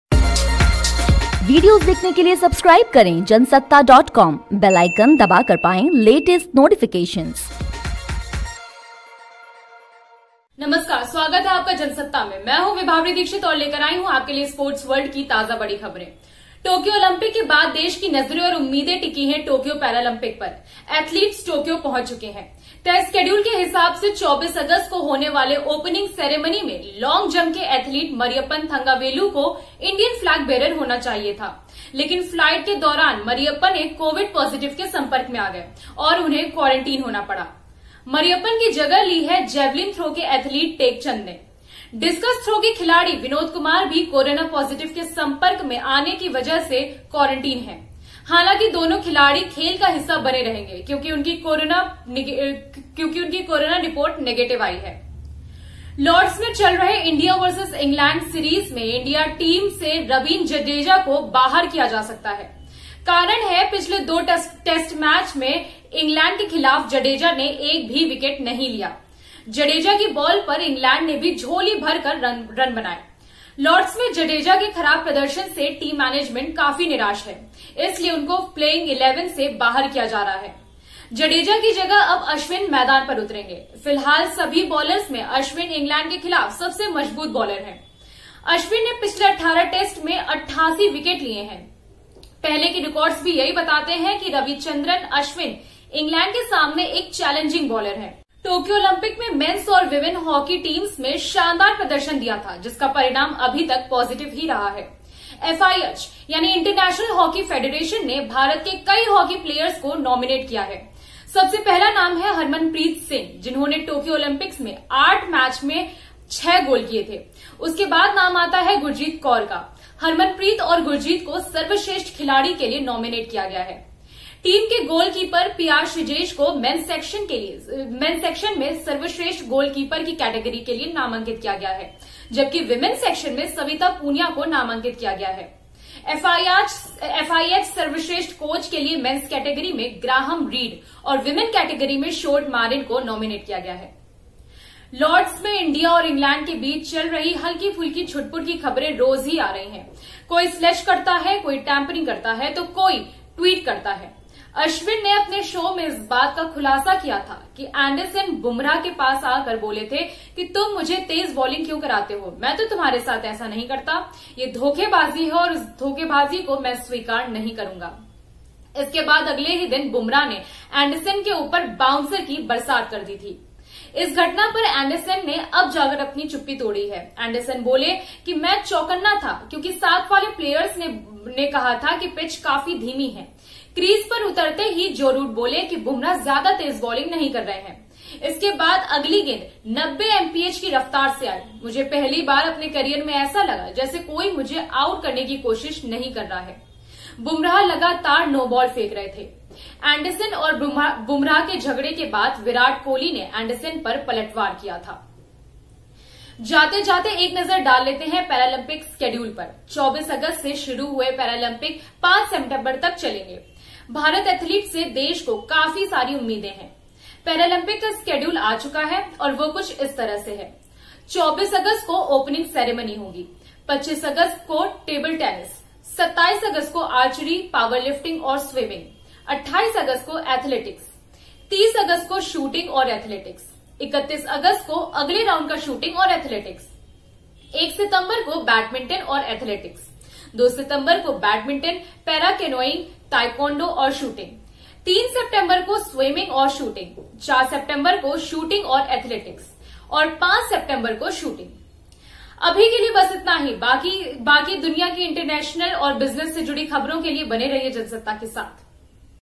पैरालम्पिक में मरियप्पन की जगह टेक चंद बने भारत के flag bearer, FIH में भारतीय खिलाड़ियों का नामांकन।
लॉर्ड्स में चल रहे india vs england सीरीज में इंडिया टीम से रविन्द्र जडेजा को बाहर किया जा सकता है। कारण है पिछले 2 टेस्ट में इंग्लैंड के खिलाफ जडेजा ने एक भी विकेट नहीं लिया। जडेजा की बॉल पर इंग्लैंड ने भी झोली भर के रन बनाए थे। लॉर्ड्स में जडेजा के खराब प्रदर्शन से टीम मैनेजमेंट निराश है इसीलिए उनको प्लेइंग 11 से बाहर किया जा रहा है। Jansatta is one of the leading hindi news channel, which provides all the latest and trending news of india and around the world.